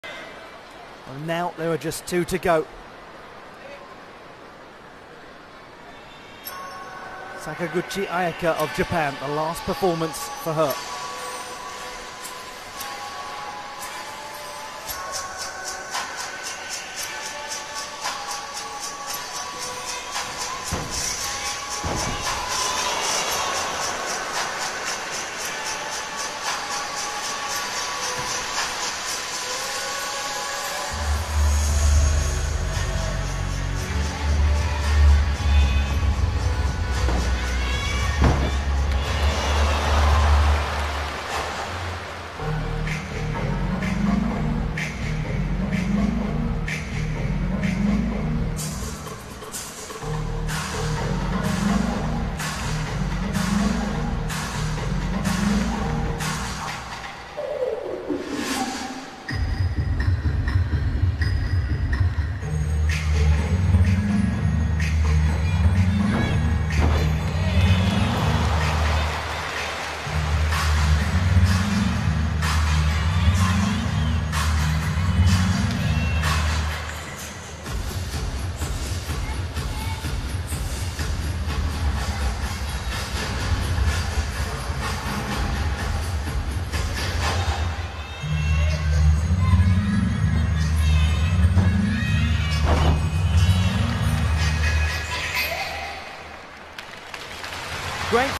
Sakaguchi Ayaka 坂口彩夏 Floor | 2023 University Games
D-score: 5.0
E-score: 8.066
Score: 13.066